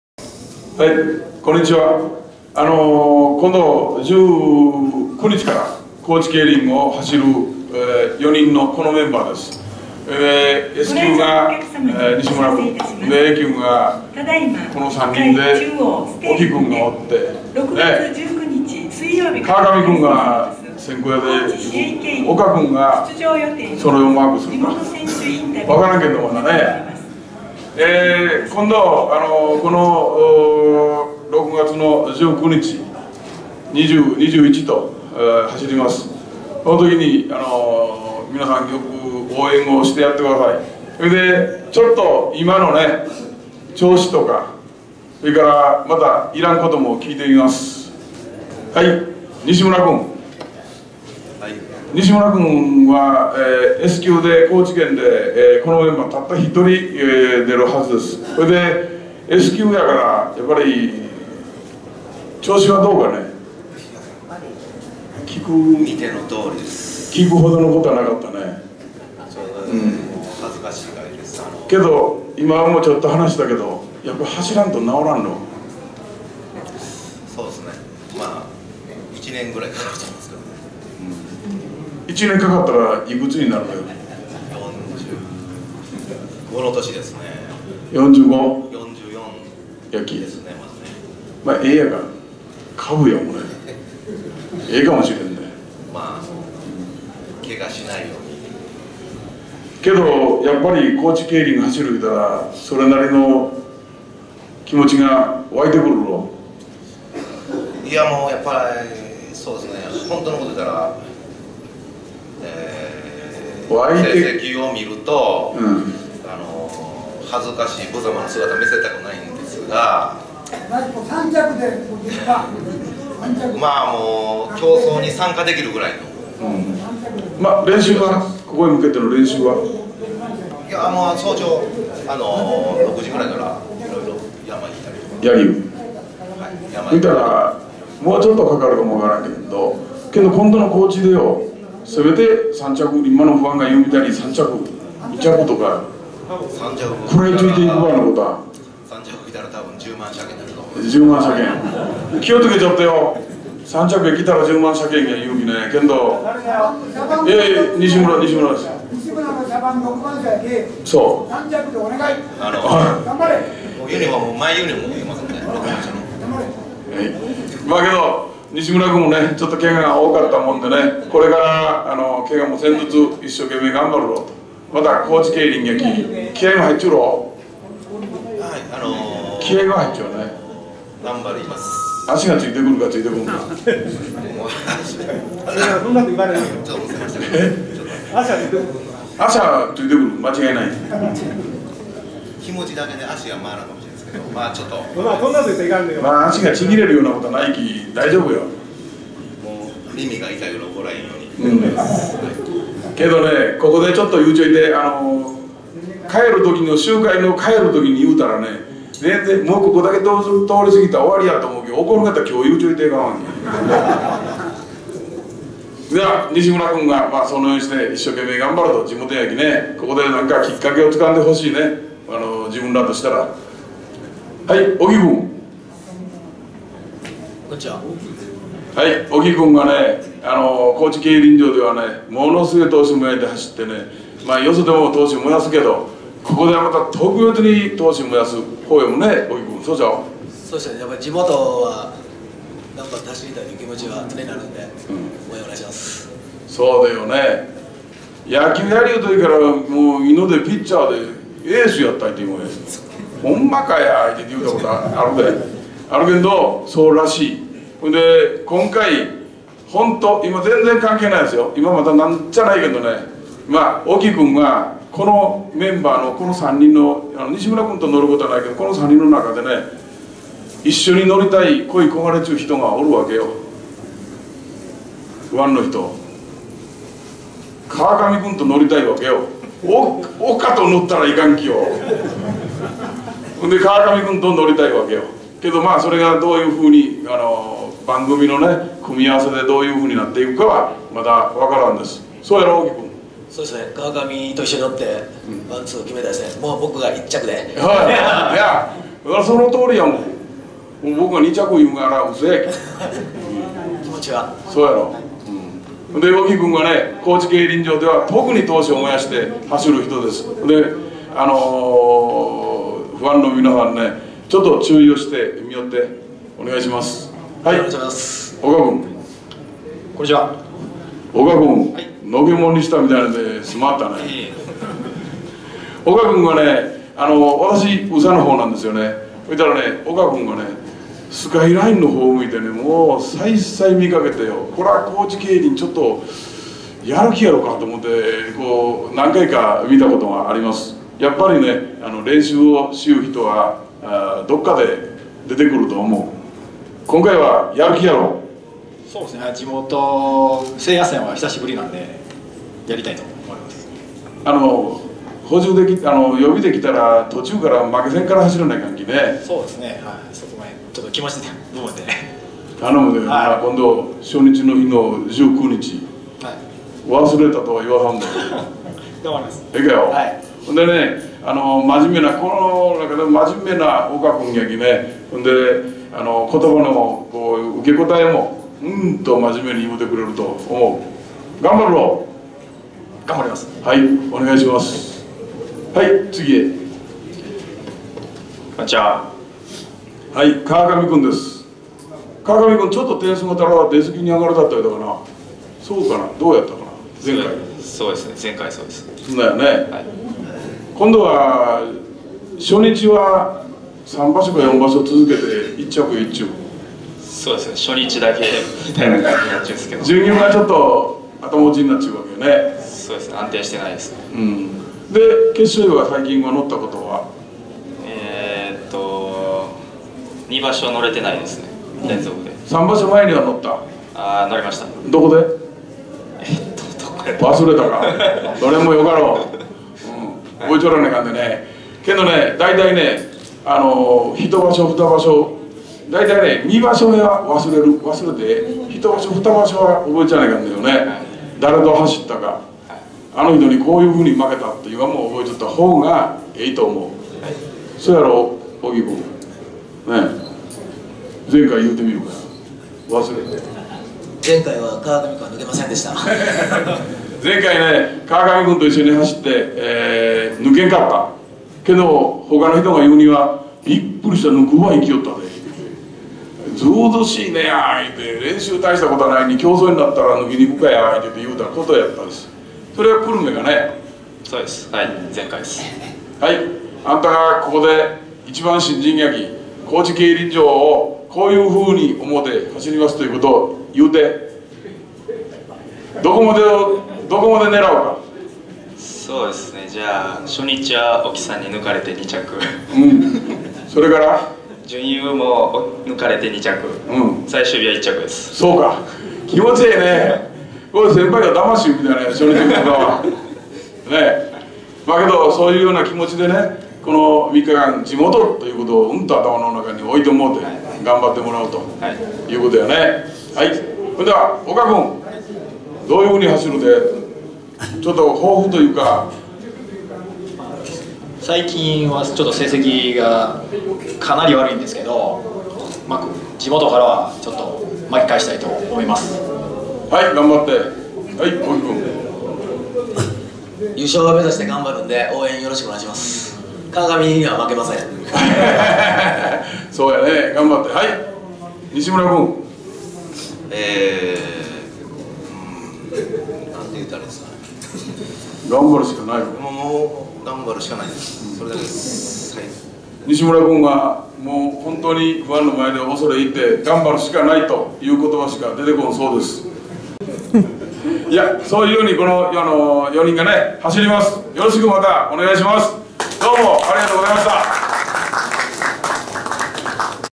6/16(日) 高知市営 第3回 前節 (FⅠ) 前日選手紹介
高知市営 第3回 前節 (FⅠ) 前日選手紹介 6/16(日)
『第32回日刊スポーツ杯争奪戦』